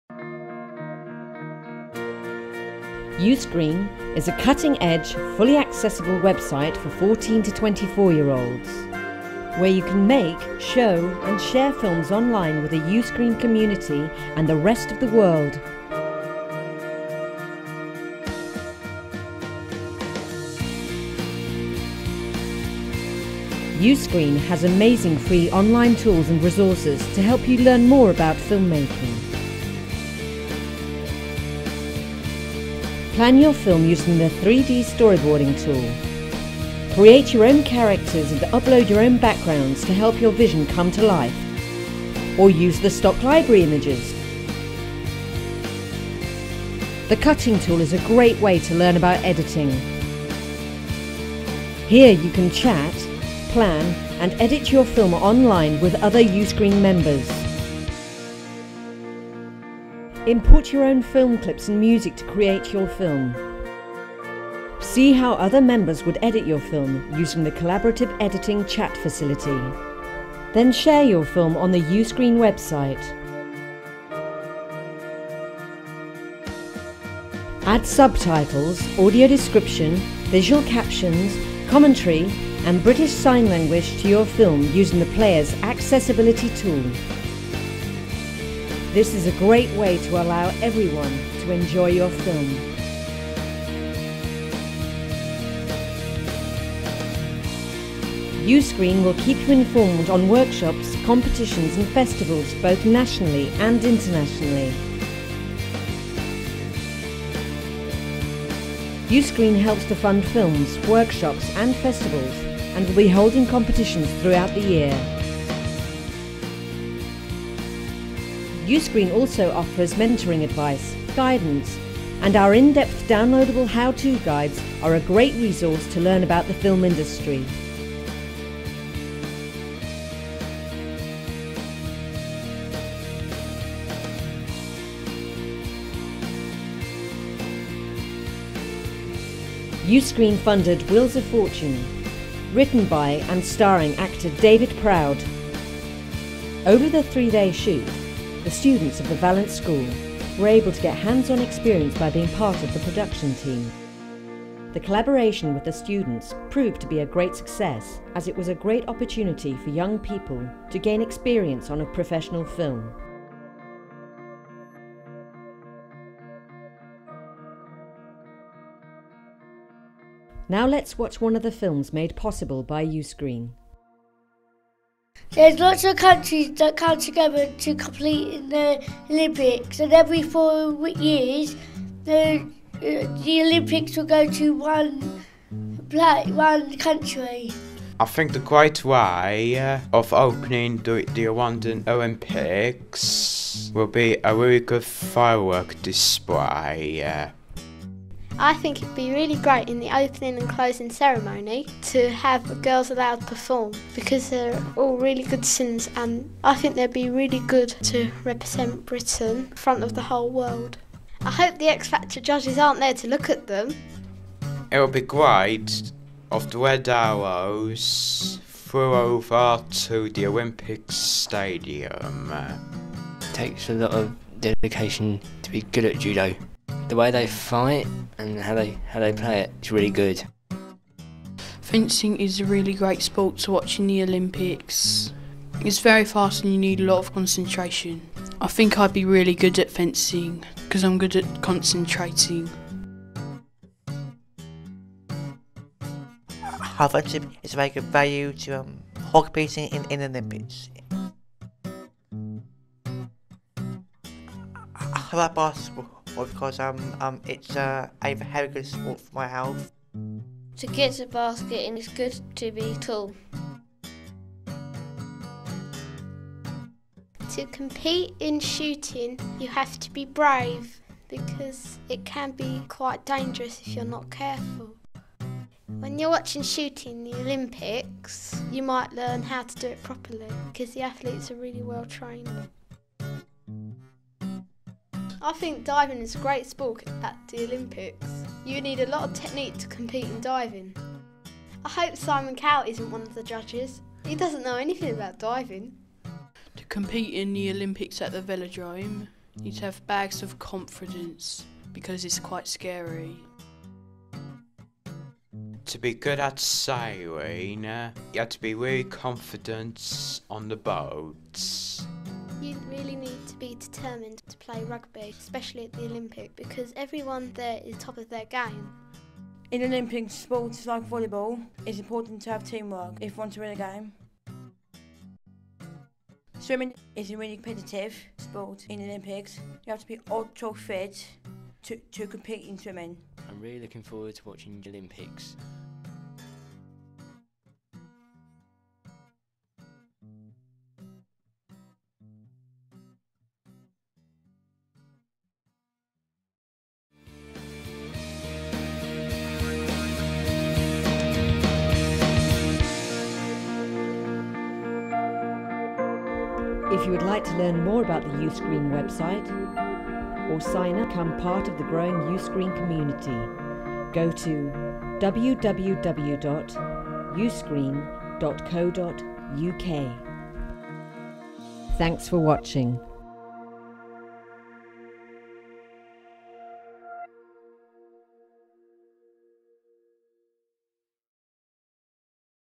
uScreen Website Promo
This film gives you an overview of how much fun you can have using the website.
Uscreen is open to all 14-24 year olds to make, show and share films.